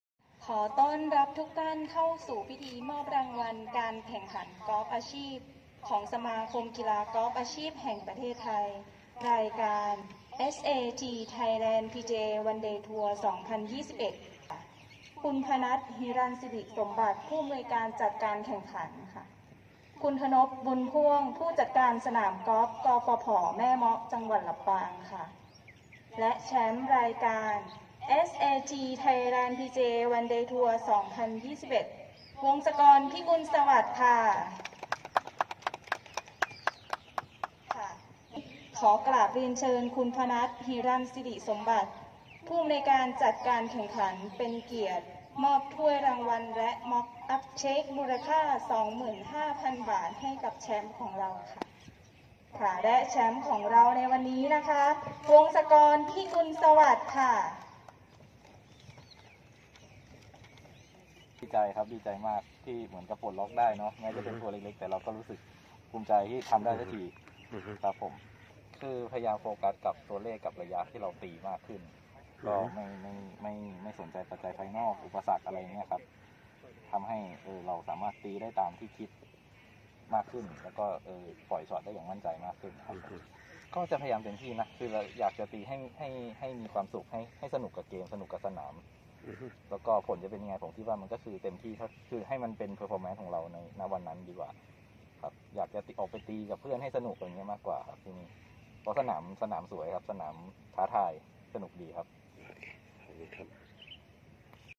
Wongsakorn PIKUNSAWAT Win 4th SAT-Thailand PGA One Day Tour 2021
ดีใจทำสำเร็จได้ซักที. วงศกร พิกุลสวัสดิ์ นักกอล์ฟวัย 28 ปีจาก โคราช เปิดใจหลังรับโทรฟี่แชมป์ และเช็คเงินสด 25,000 บาท หลังปิดเกมด้วยสกอร์ 5 อันเดอร์พาร์ 65 คว้าแชมป์ ไทยแลนด์ พีจีเอ วันเดย์ ทัวร์ สนามที่ 4 แข่งที่สนาม แม่เมาะ กอล์ฟ คอร์ส จ.ลำปาง แบบพาร์ 70 ระยะ 6,781 หลา เมื่อวันจันทร์ที่ 22 พ.ย.ที่ผ่านมา มาฟังความรู้สึกของ วงศกร กันว่าคิดอย่างไรกับการครองแชมป์ในครั้งนี้